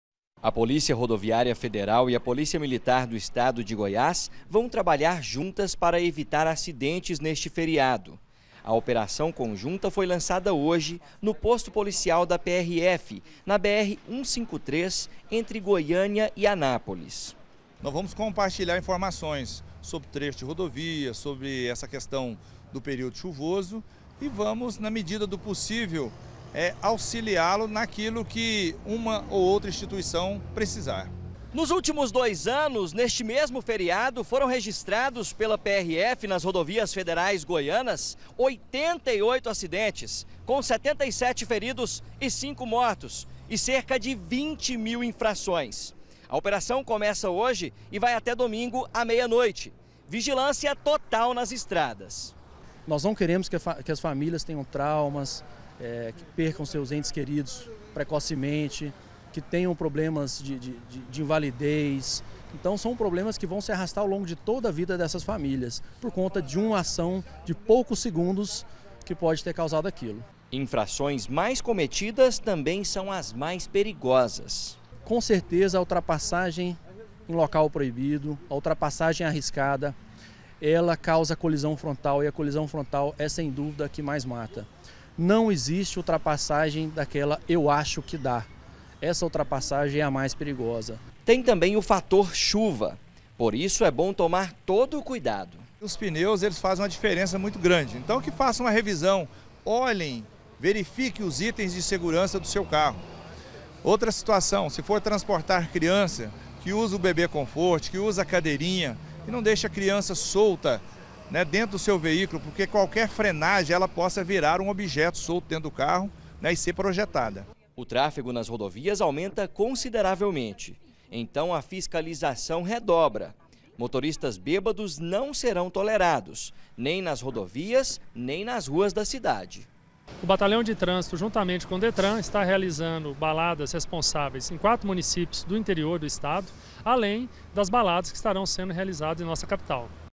VIGILÂNCIA NAS ESTRADAS: PRF E PM REFORÇAM FISCALIZAÇÃO NO FERIADO
Fiscalização redobrada nas rodovias do estado durante o feriadão. A polícia rodoviária federal e a polícia militar vão trabalhar em conjunto para conter infrações de trânsito.